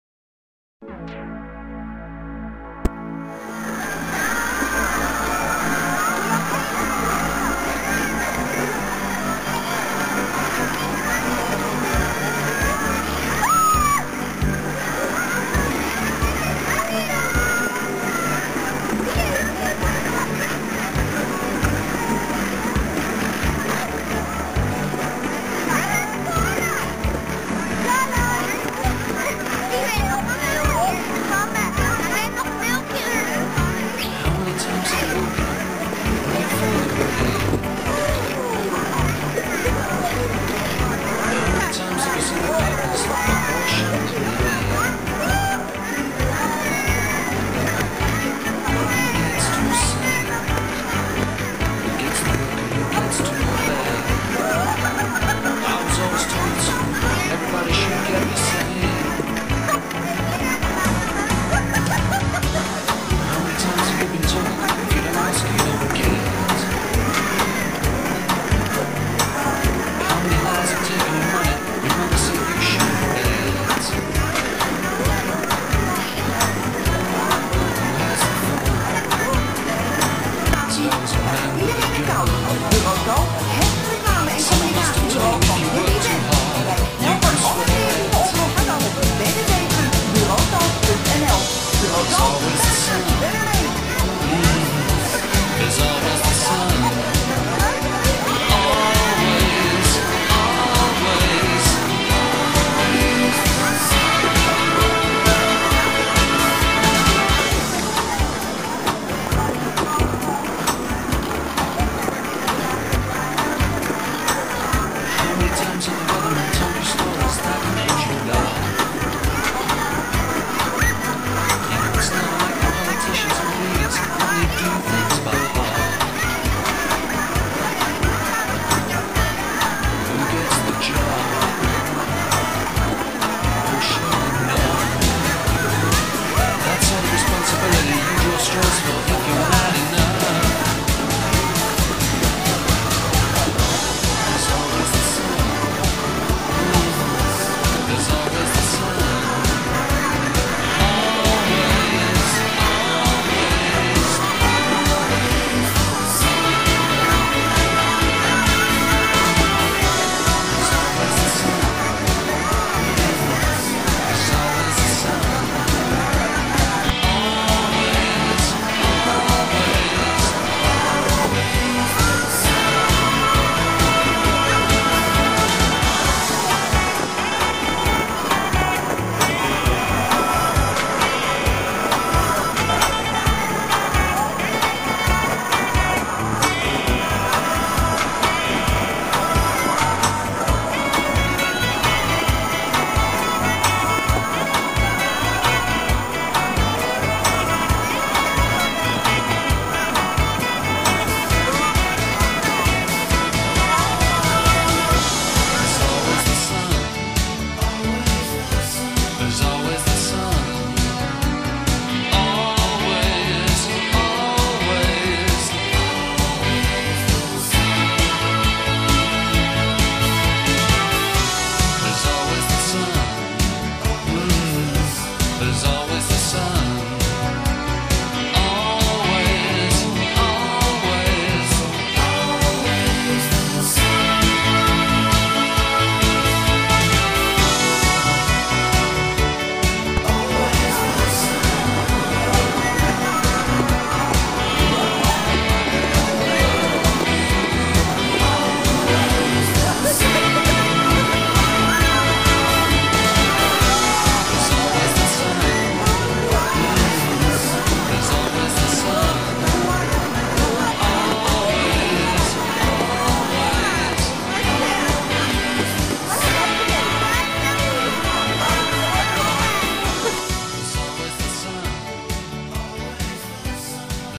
schaatsen op het rijnplein
Marcel,Nettie,Melissa en Dylan zijn lekker aan het schaatsen op de schaatsbaan in Alphen aan den Rijn